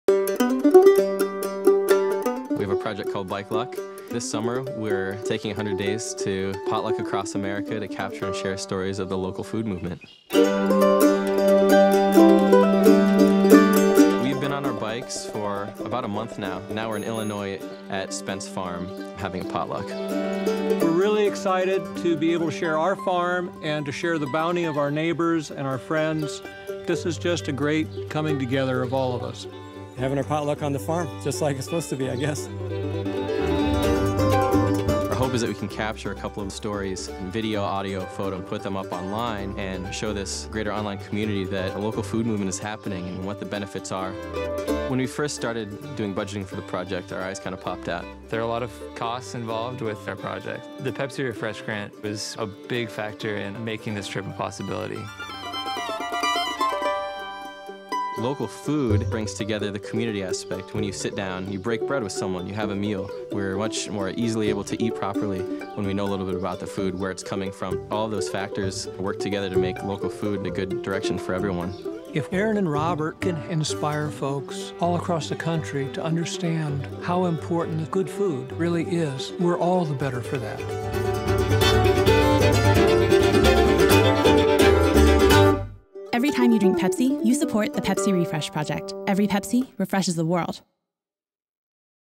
Pepsi Refresh Project Homegrown Heroes: Bikeloc
Robert DuBois and Aaron Zueckbike across America for 100 days to share the local food movement through potlucks on various farms.